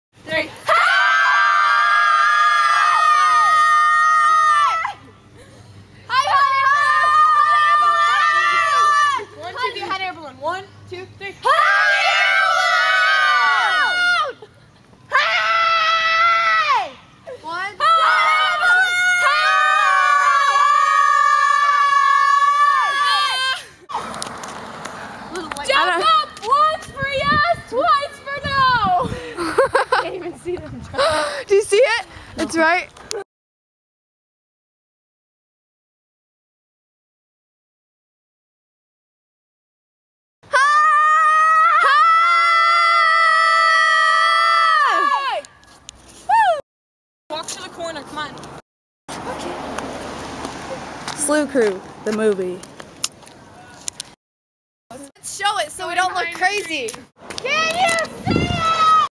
YELLING TO HOT-AIR BALLOONS
This Video Features:

Tara
Tina
Jess
Britni
Kellye

"HAHAHHA. We are crack-ups!"

Filmed: May 28th 2010

TheSlewcrew
Britni|Kellye|Tina|Jess|Tara|Ali